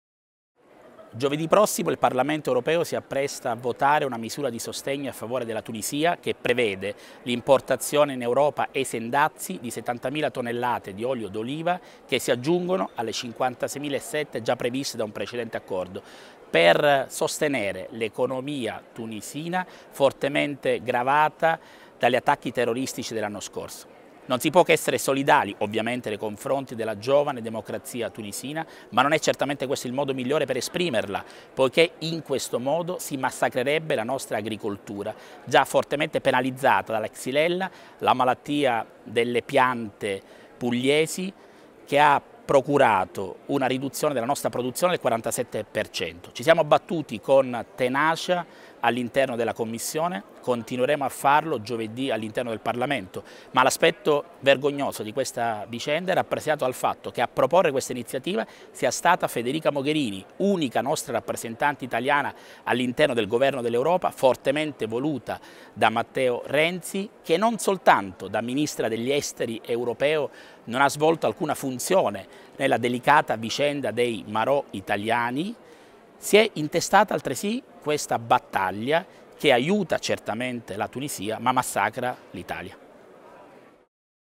No all'olio d'oliva dalla Tunisia
La nostra agricoltura, già in difficoltà, ha bisogno di tutto tranne che di scellerate politiche provenienti da Bruxelles. Giovedì, nella seduta plenaria del Parlamento europeo, ci batteremo ancora una volta contro l’importazione dell’olio d’oliva dalla Tunisia.